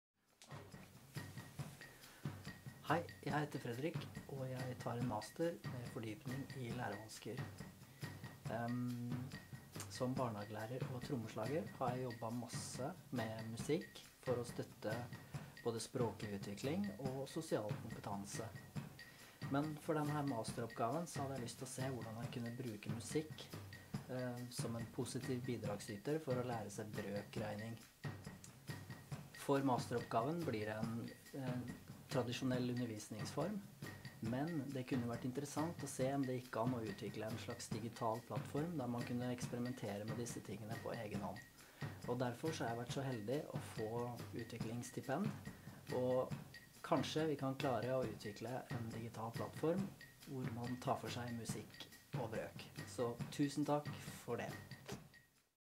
Kan du høre brøken?
Fredrik Brarud, student ved Institutt for spesialpedagogikk, UiO skriver masteroppgave om en musisk tilnærming til brøkregning.